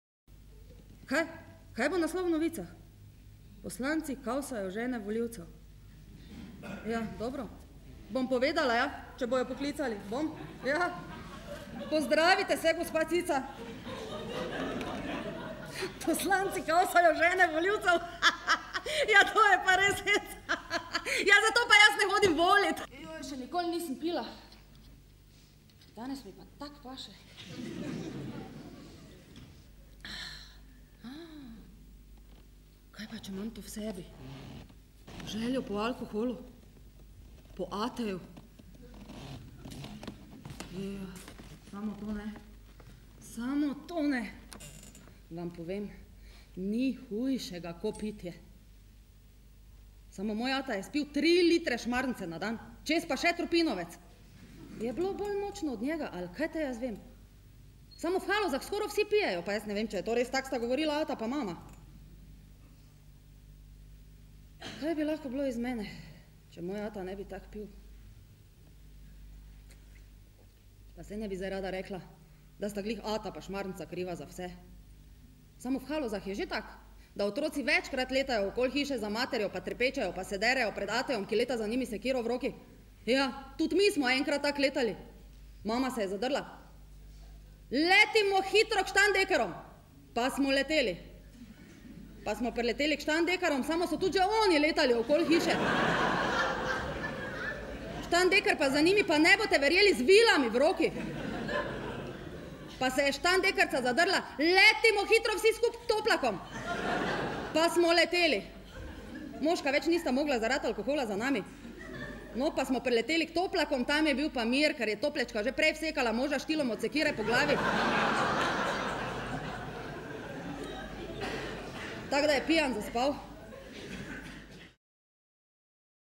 Tone Partljič - Čistilka Marija
Monokomedija
Igra: Mojca Partljič
Režiser: Vito Taufer
Snemalca: Tone Stojko, Simon Stojko Falk